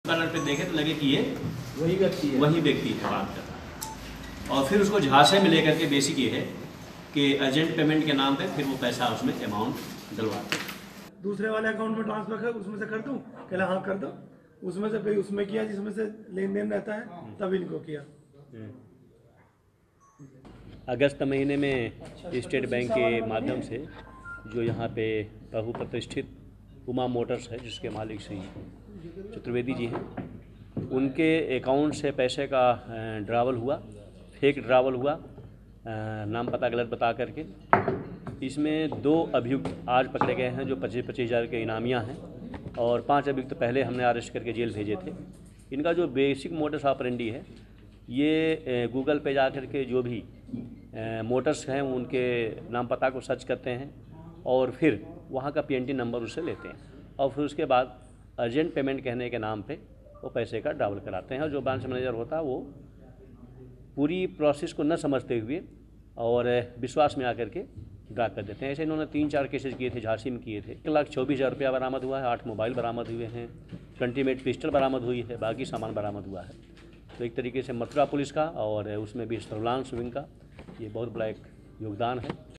मथुरा : लाखों की ठगी करने वाले गैंग का पर्दाफाश, दो गिरफ्तार
मथुरा : पुलिस ने ठगी करने वाले अंतरराज्यीय गैंग के मास्टरमाइंड सहित दो लोगो को गिरफ्तार किया है. ये गैंग मथुरा के प्रतिष्ठित उद्योगपति के साथ भी लाखों रुपये का फ्रॉड कर चुका है. पकड़े गए लोगों के पास से अवैध हथियारों सहित कार और मोबाइल बरामद किए गए है.